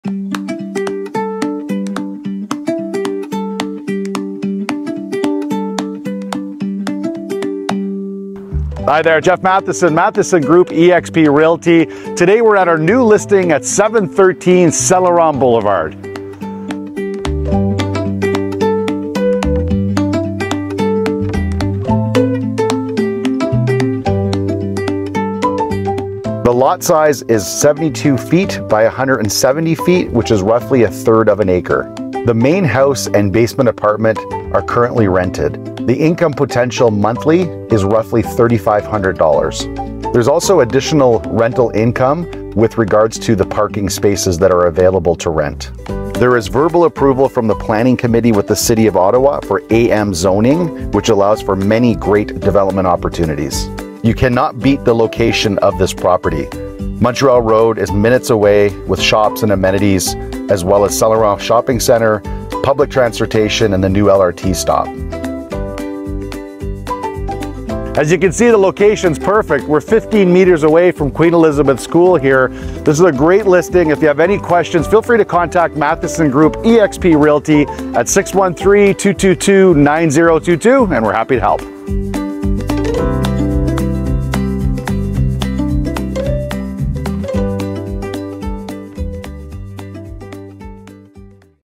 For Sale | 713 St Laurent Blvd | Ottawa, ON
Outstanding opportunity located only 5-10 mins to downtown. Public transit, parks, schools, churches, restaurants, shopping, & more just steps away. Currently zoned IA1-Institutional but pre-consultation application in place for AM Zoning with a verbal approval from City of Ottawa Planning Committee.   Lot Size 73' x 170.86' (over 12,000 sq.ft). The home is currently a 3 bed, 3 bath structure with 2 covered porches and an attached workshop area. Main level features a large country style eat in kitchen, spacious living area, great room, powder room, laundry & inside access to workshop. Upper level w/3 large bedrooms and full bath. Lower level apartment with 1 bedroom, full bathroom, kitchen, laundry, living space and 2 separate exterior entrances.  Potential additional income from renting parking spaces to nearby businesses.